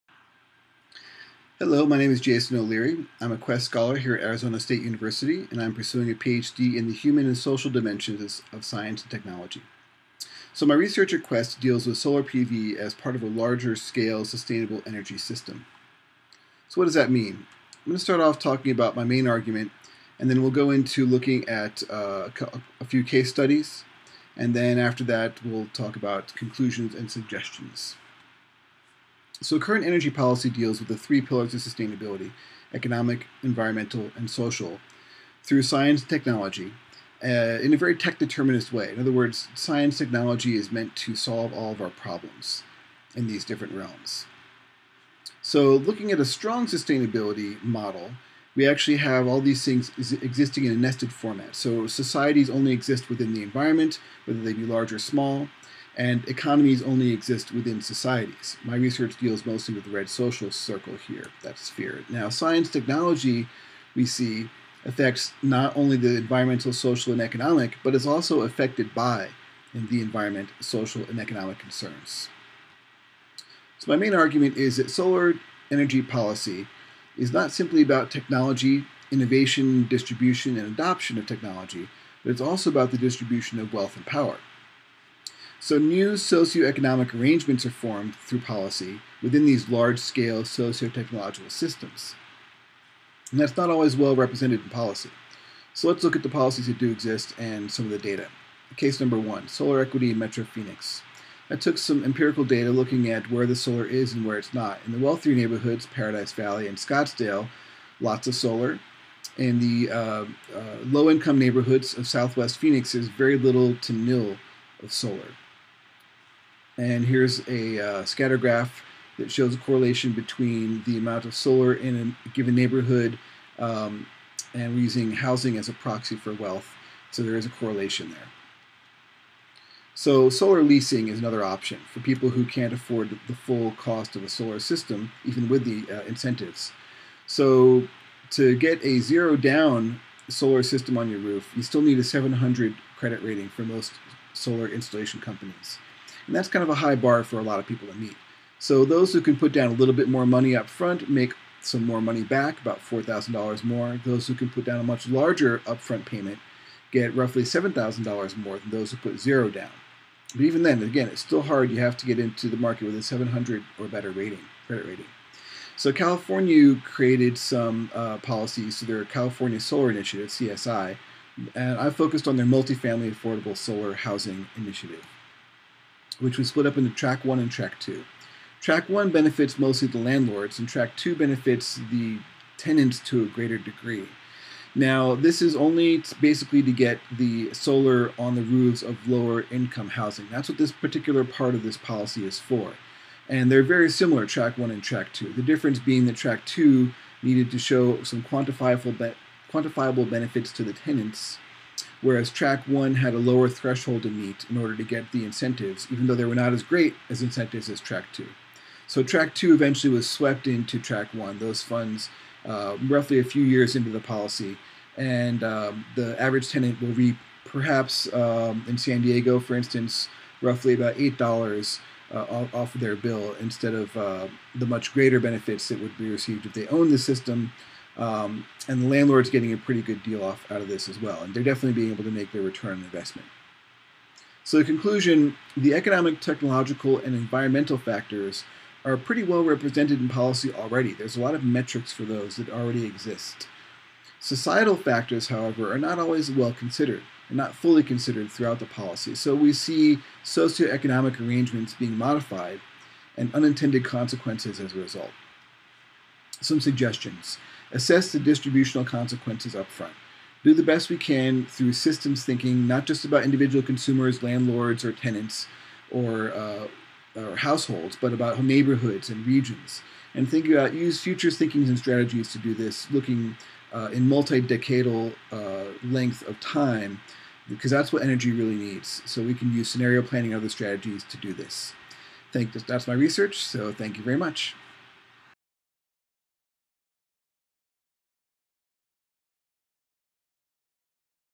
Solar PV: Sustainable Energy Systems
QESST graduate student Jason O'Leary presents a short summary of his work on sustainable PV systems.